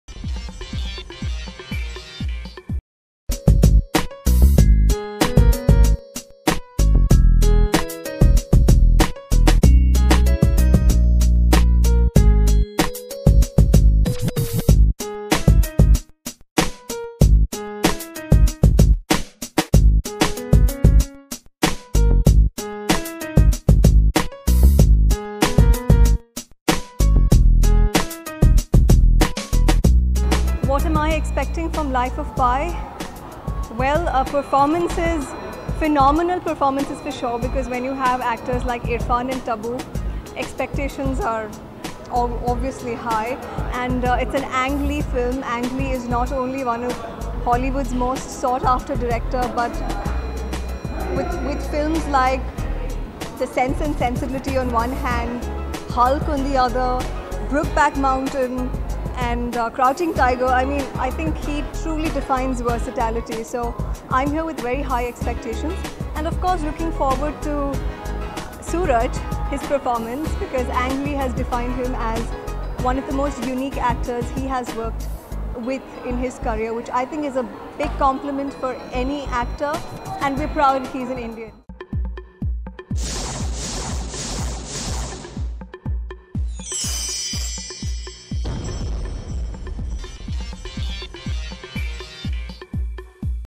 Amrita Rao In Black Sleeveless GOWN @ Life Of Pi Movie Screening
Actors Irrfan Khan and Tabu, who will be seen in 'Life Of Pi', spoke about how excited they were about the movie. Other celebs like Naseeruddin Shah, Kabir Khan, Gauri Shinde and David Dhawan also gushed about the trailer of the film and seemed quite excited to watch it.